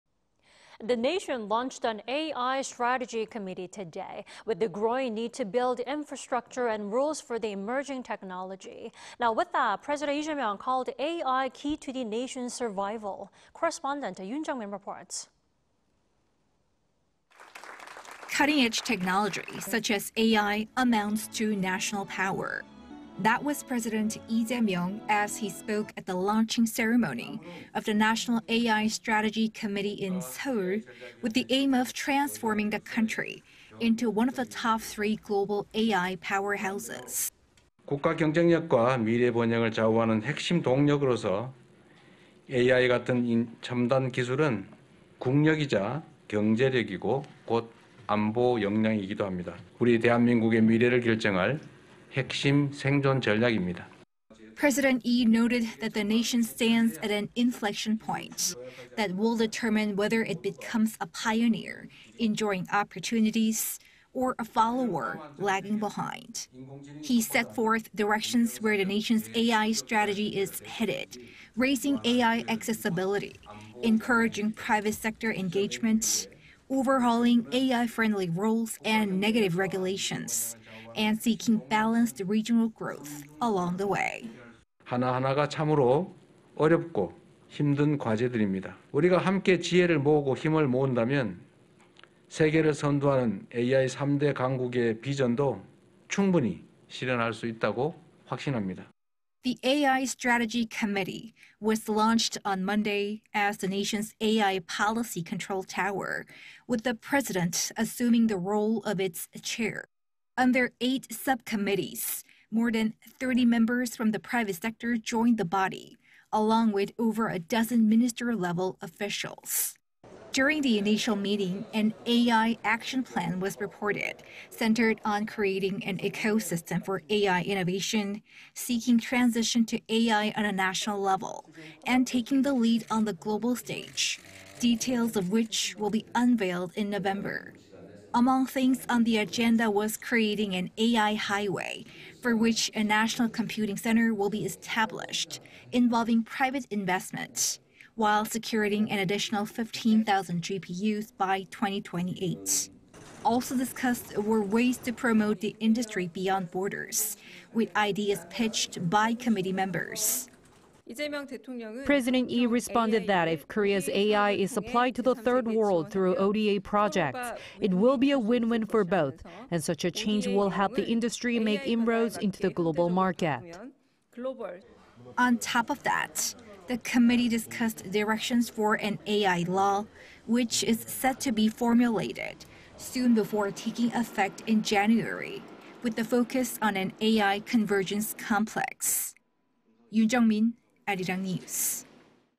Lee calls AI "national power" as National AI Strategy Committee launched
李대통령 "AI 기술이 국력"… 국가인공지능전략위원회 출범

The nation launched an AI strategy committee today, with the growing need to build infrastructure and rules for the emerging technology.
With that, President Lee Jae Myung called AI key to the nation's survival.
Correspondent Yoon Jung-min reports.

Cutting-edge technology, such as AI, amounts to national power.
That was President Lee Jae Myung as he spoke at the launching ceremony of the National AI Strategy Committee in Seoul, with the aim of transforming the country into one of the top three global AI powerhouses.

"As a key driver that determines the nation's competitiveness and future prosperity, advanced technologies, such as AI, mean national and economic power, as well as security capability. It is a key survival strategy that will determine the future of the Republic of Korea."

President Lee noted that the nation stands at an "inflection point" that will determine whether it becomes a pioneer enjoying opportunities or a follower lagging behind.
He set forth directions where the nation's AI strategy is headed: raising AI accessibility, encouraging private sector engagement, overhauling "AI-friendly" rules and "negative regulations," and seeking balanced regional growth along the way.

"Each and every task is challenging. If we put wisdom and strength together, I'm confident that we can realize the vision of becoming one of the world's leading AI powerhouses."

The AI strategy committee was launched on Monday as the nation's AI policy control tower, with the president assuming the role of its chair.
Under eight subcommittees, more than 30 members from the private sector joined the body, along with over a dozen minister-level officials.

During the initial meeting, an "AI action plan" was reported, centered on creating an ecosystem for AI innovation, seeking transition to AI on a national level, and taking the lead on the global stage, details of which will be unveiled in November.

Among things on the agenda was creating an "AI highway," for which a national computing center will be established involving private investment, while securing an additional 15-thousand GPUs by 2028.

Also discussed were ways to promote the industry beyond borders, with ideas pitched by committee members.

"President Lee responded that if Korea's AI is supplied to the third world through ODA projects, it will be a win-win for both, and such a change will help the industry make inroads into the global market."

On top of that, the committee discussed directions for an AI law, which is set to be formulated soon before taking effect in January, with the focus on an AI convergence complex.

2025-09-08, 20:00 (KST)